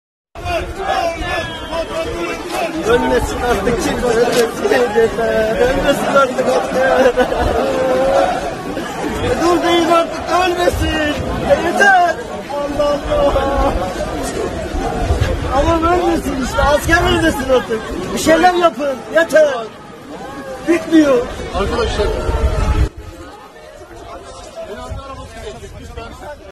Artı Gerçek -
Kardeşini İdlib'de kaybetti, cenaze töreninde konuştu: Asker ölmesin artık, bir şeyler yapın